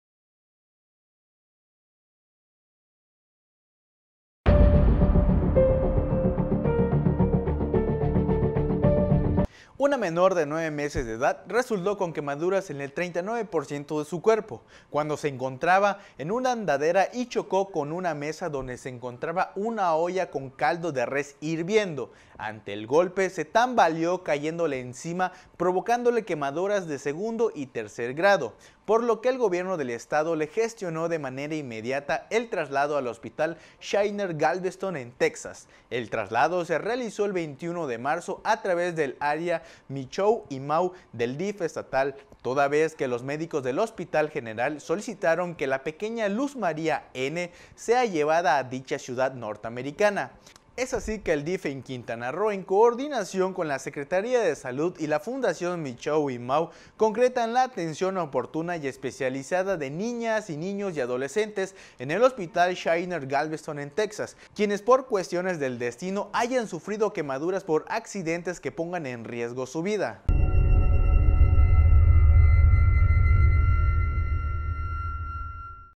MENOR SE QUEMA CON CALDO DE RES; LA LLEVAN A TEXAS
Entérate de las noticias “EN CORTO...MENOR SE QUEMA CON CALDO DE RES; LA LLEVAN A TEXAS …